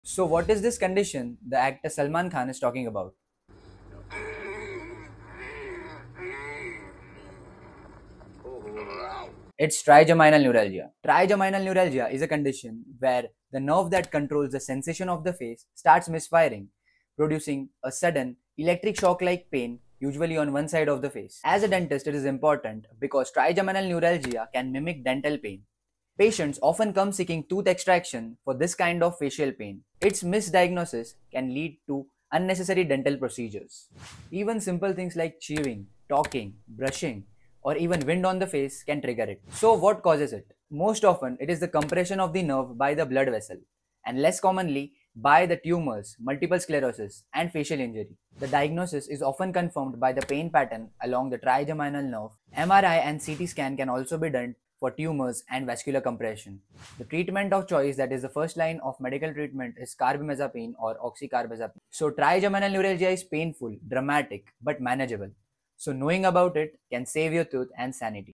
Everything you need to know about SUICIDE DISEASE - Trigeminal Neuralgia ✨
Often called as SUICIDE DISEASE , here’s everything you need to know about Trigeminal Neuralgia 💀

🧠 It involves the Trigeminal nerve (5th cranial nerve), which carries sensation from the face to the brain.

💥 Symptoms:
 • Sudden, electric shock–like facial pain
 • Usually triggered by chewing, talking, brushing teeth, or even a light touch
 • Pain lasts seconds to minutes but can recur multiple times a day

❓ Causes:
 • Compression of the trigeminal nerve by a blood vessel
 • Rarely, multiple sclerosis or tumors

💊 Management:
 • First-line: Anti-seizure medicines (e.g., Carbamazepine)
 • In resistant cases: Nerve blocks, microvascular decompression, or radiosurgery